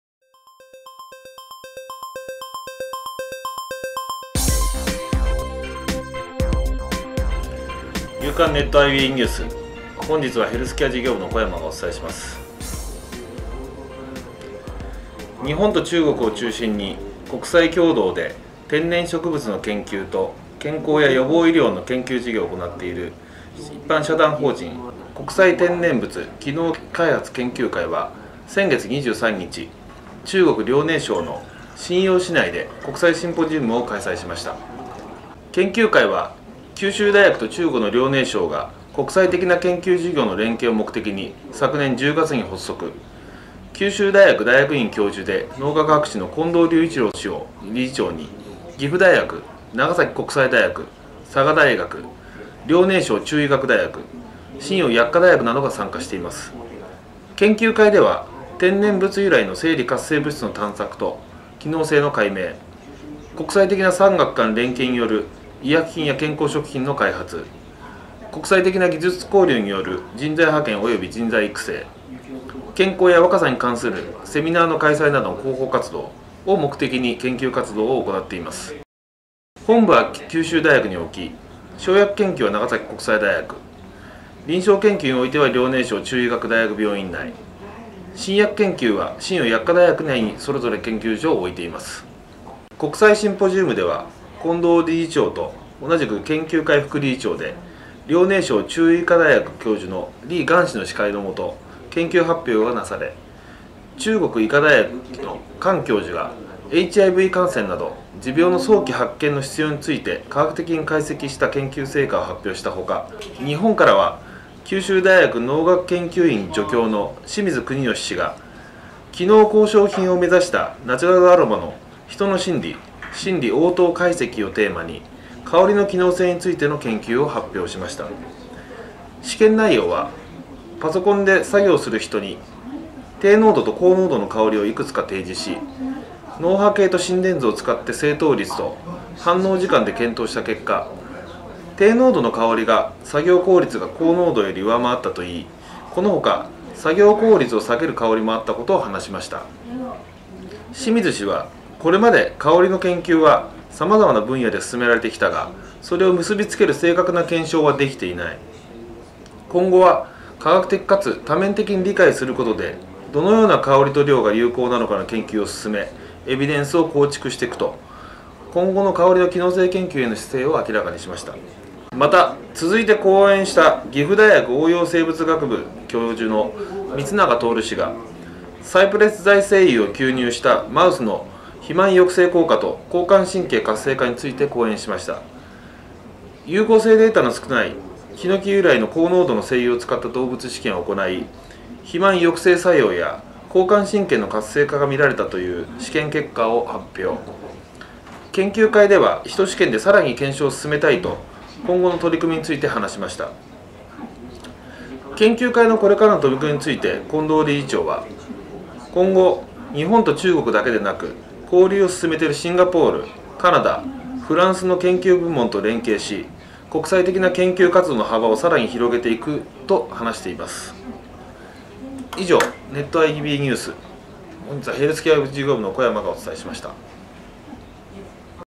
【夕刊 NET-IB】天然物の機能性解明へ～国際シンポジウムを開催
［更新：2011年10月11日］
　日本と中国の研究者が共同で天然植物を通じて健康や予防医療の研究事業を展開する一般社団法人 国際天然物機能開発研究会は2011年９月23日、中国・遼寧省瀋陽市内で国際シンポジウムを開催しました。
◆詳しくはネットIBへ

｜NetIB-NEWS｜－－－－－－－－－－
経済・政治・企業などのニュースを配信！
"ネットアイビーニュース"